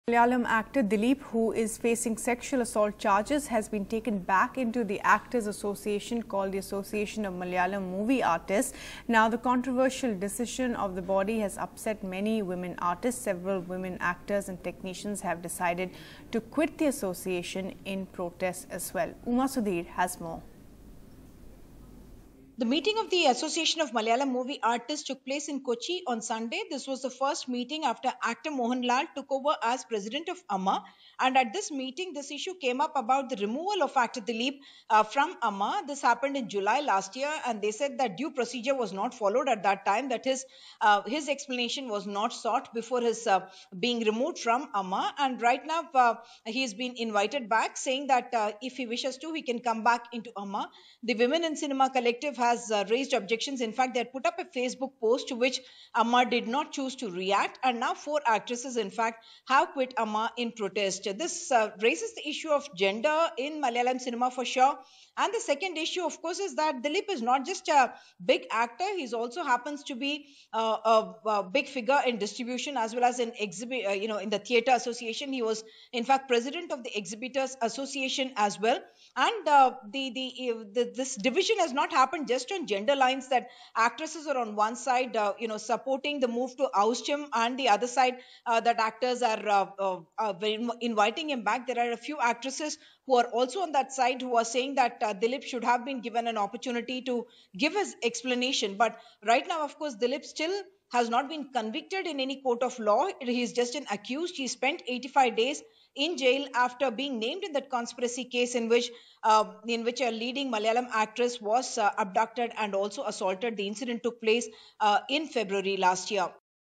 Actor Dileep Returns To Malayalam Film Body, 4 Actresses Quit In Revolt
The row over the reinstatement of accused actor Dileep into an artistes body took a turn on Wednesday with four leading actresses, including the kidnap victim, resigning from the Association of Malayalam Movie Actors. Dileep is an accused in the February 2017 actress kidnapping case which took place in Kochi, that landed him in a Kerala jail for 85 days.